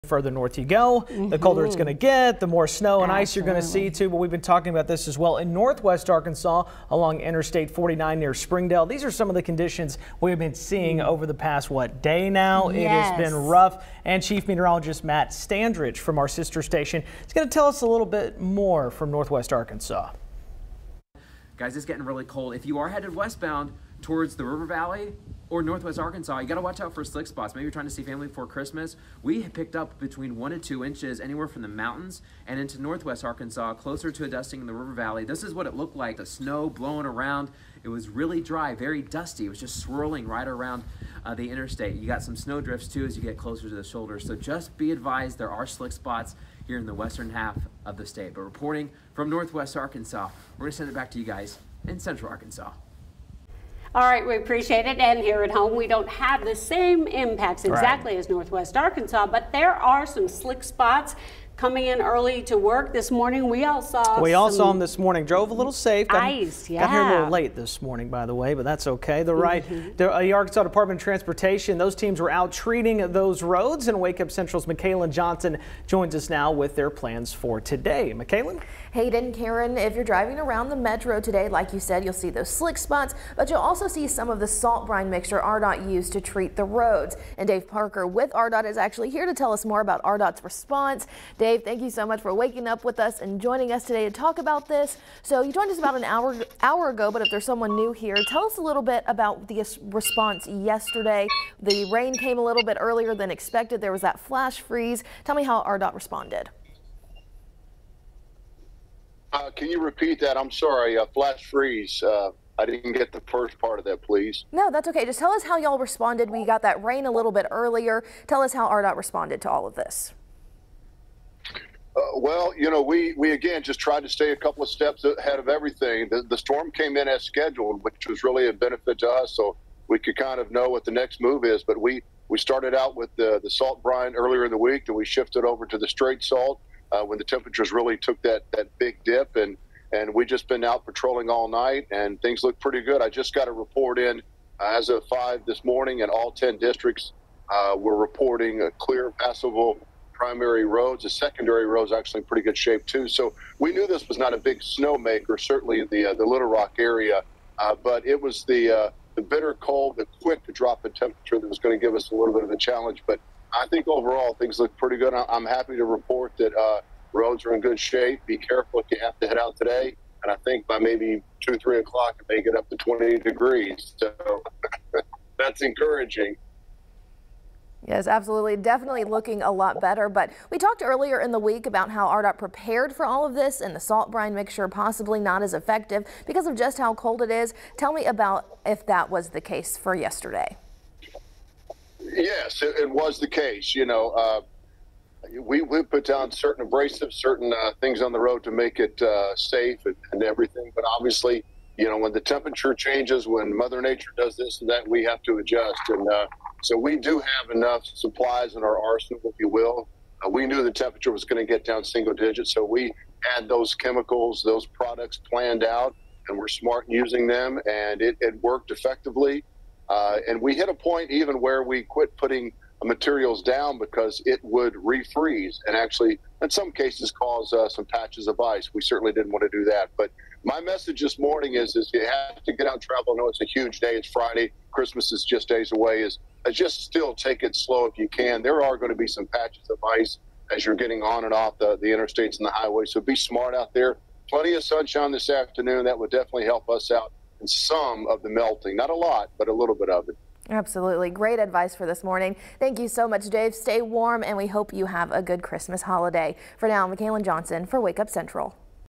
How winter weather impacted Arkansas roads
Winter weather moved into the Natural State on Thursday night, impacting several parts of Arkansas.